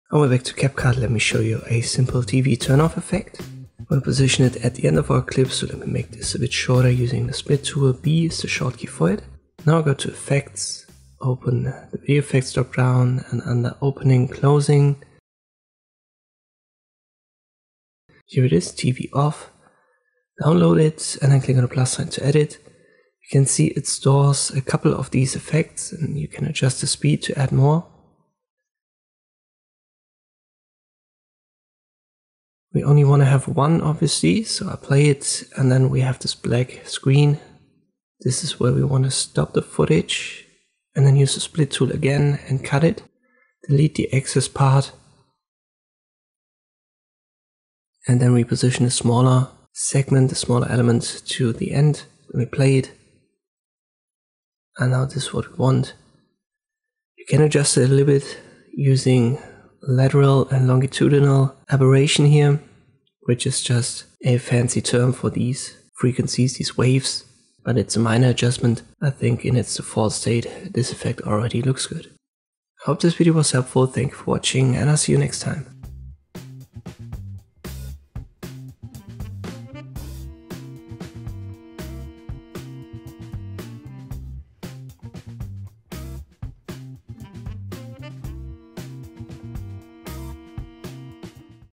How to Create a TV Turn Off Effect in CapCut
In this tutorial I'll show you how to create a tv turn off effect in CapCut.